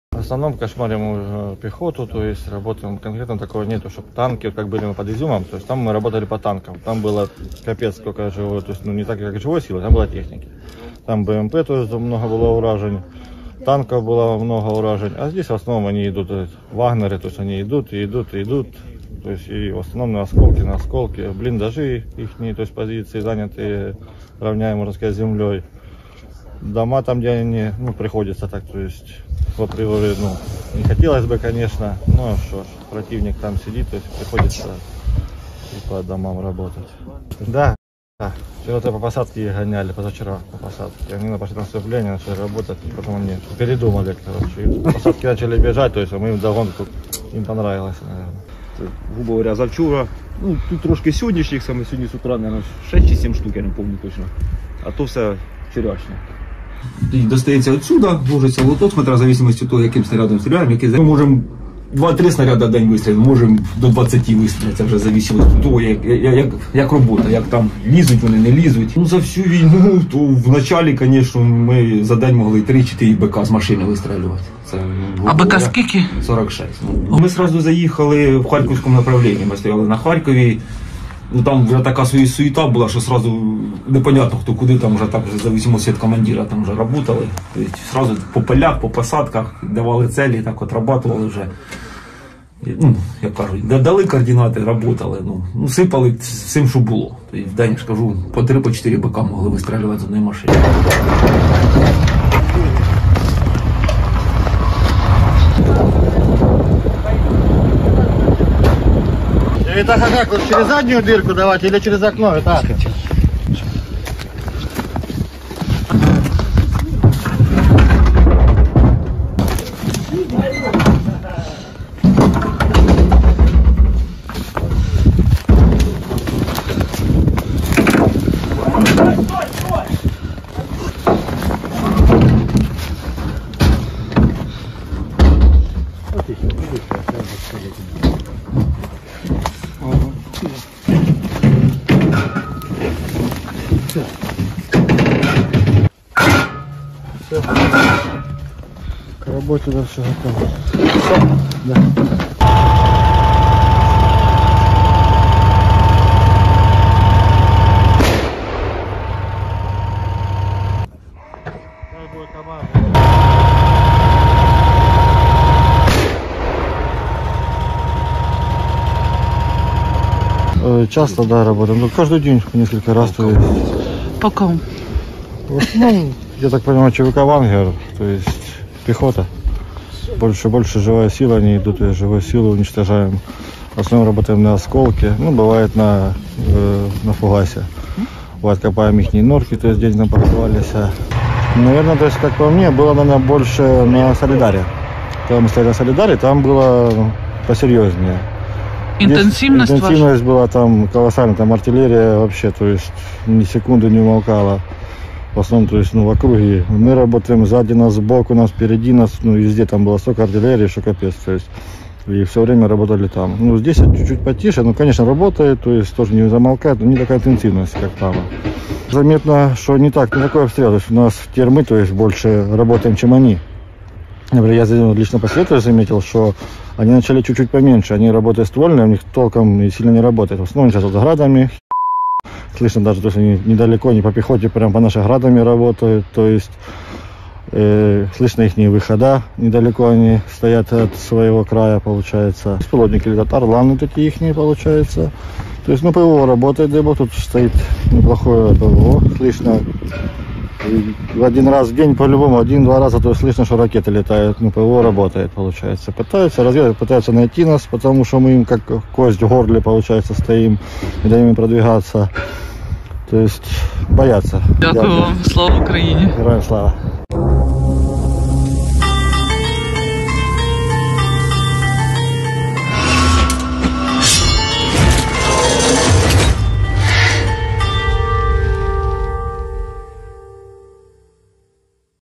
Як артилеристи ЗСУ працюють у БАХМУТІ / Героїчна робота воїнів
Артилеристи батареї 93 бригади ЗСУ "Холодний Яр" захищають піхоту на Бахмутському напрямку.